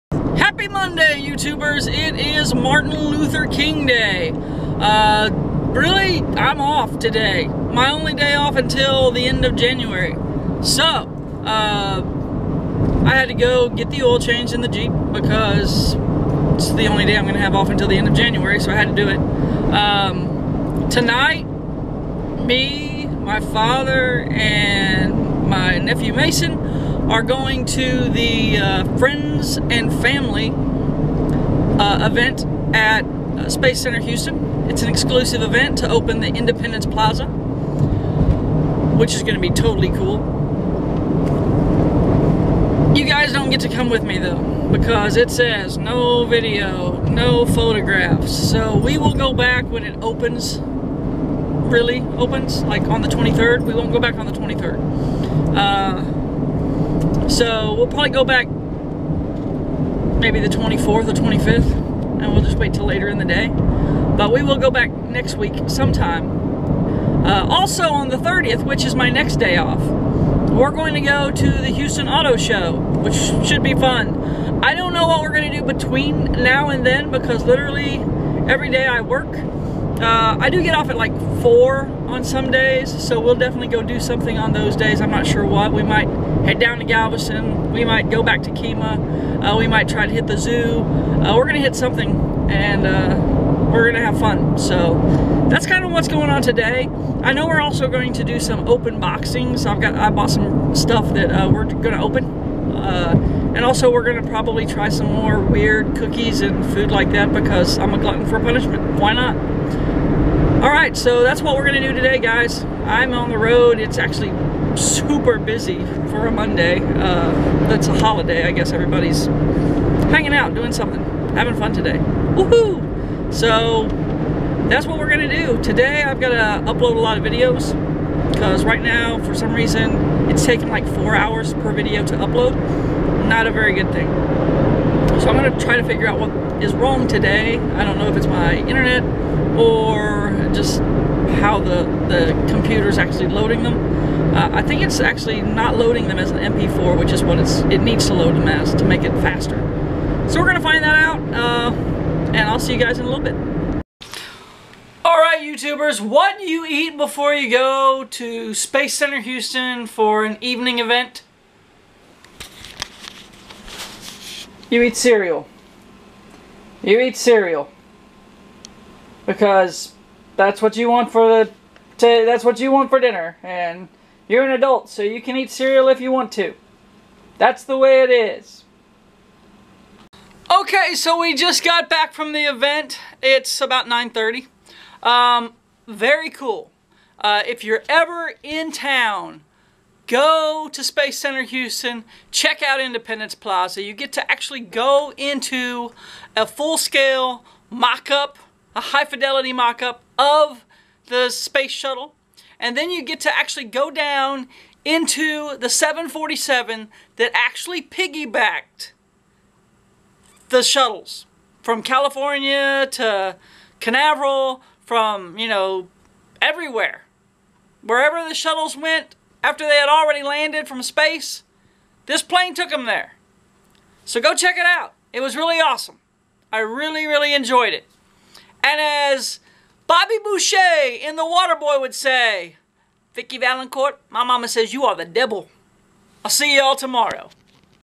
Oil Change, Cereal for Dinner and Independence Plaza Jan 18 2016
On my only off day I get an oil change, have some cereal for dinner and go check out a sneak preview on Space Center Houstons Independence Plaza. Life is good!